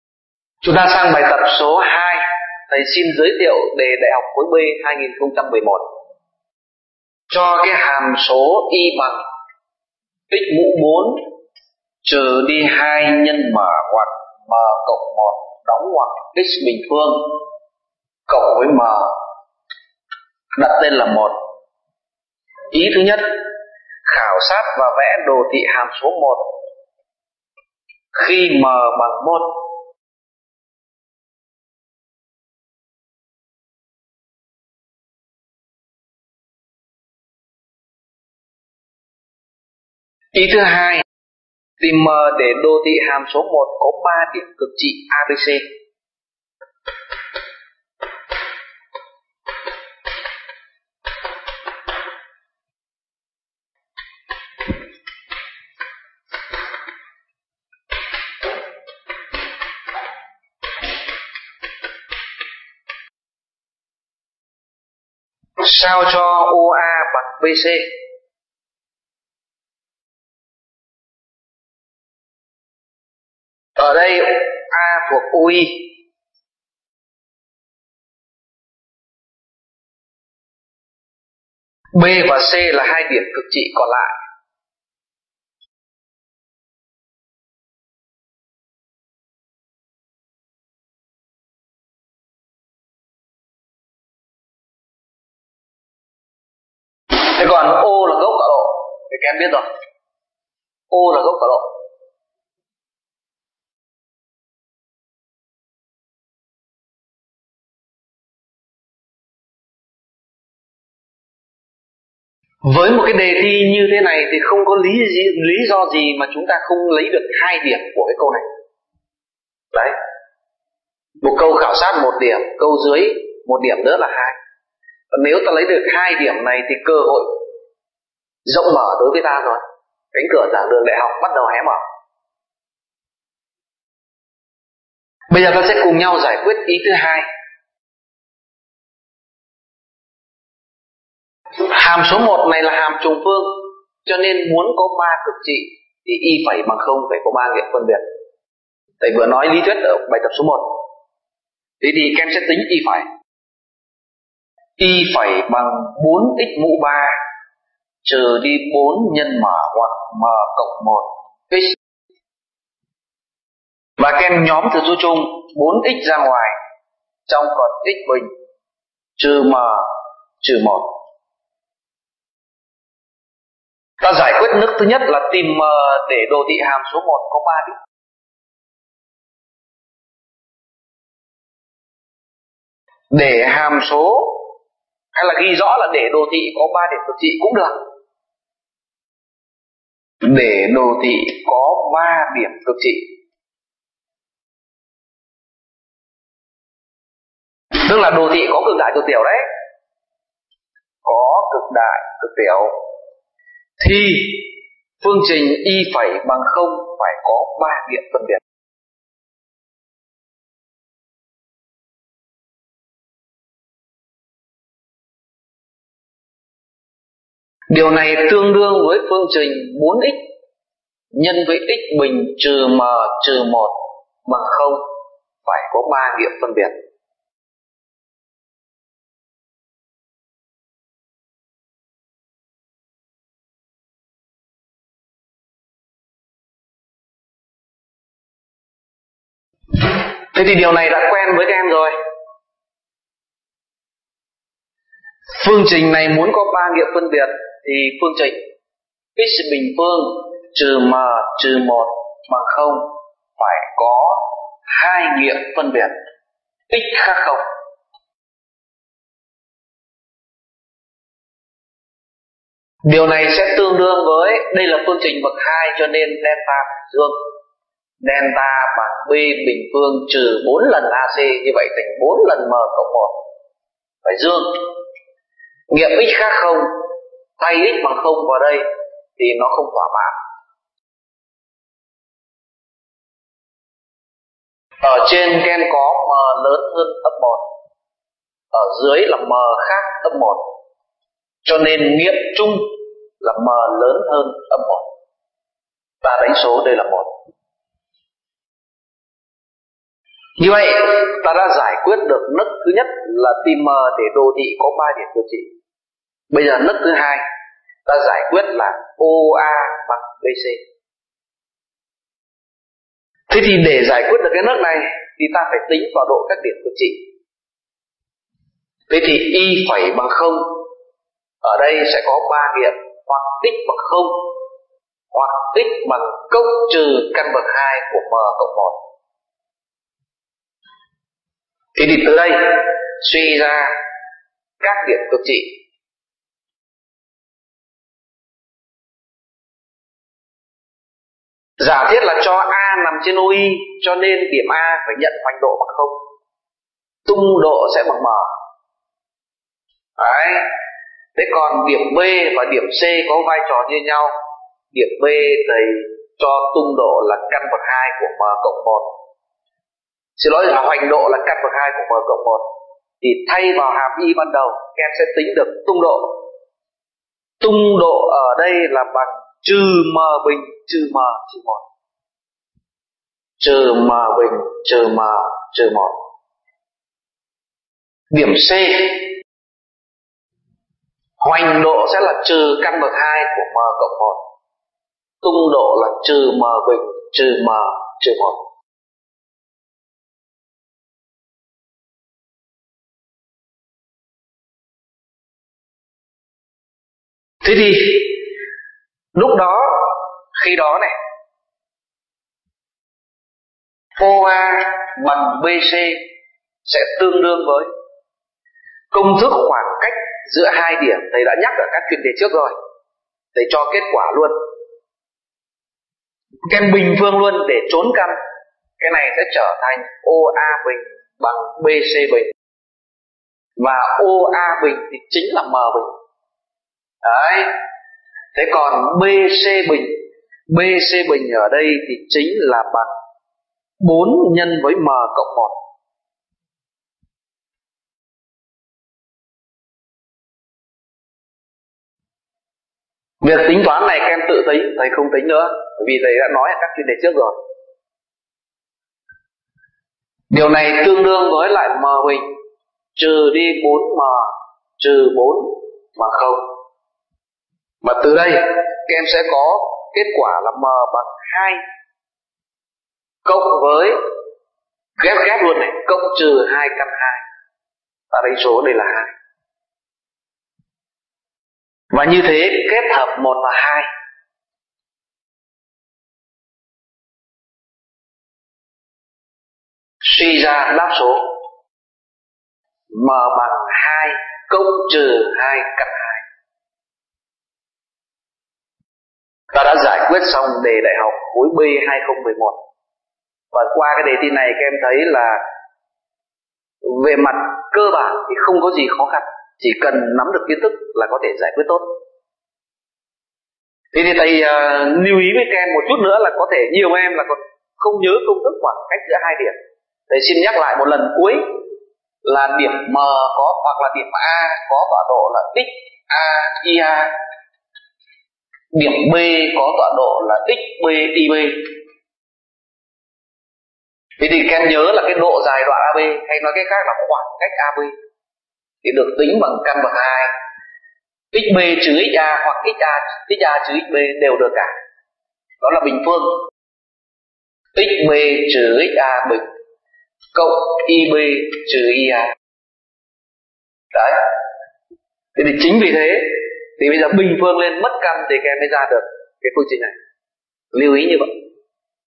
[Hocmai.vn] Cực trị hàm trùng phương - phần 4/6
Video bài giảng thuộc bài giảng tham khảo miễn phí khóa luyện thi đại học môn Toán do thầy Lê Bá Trần Phương giảng dạy. để biết thêm thông tin chi tiết!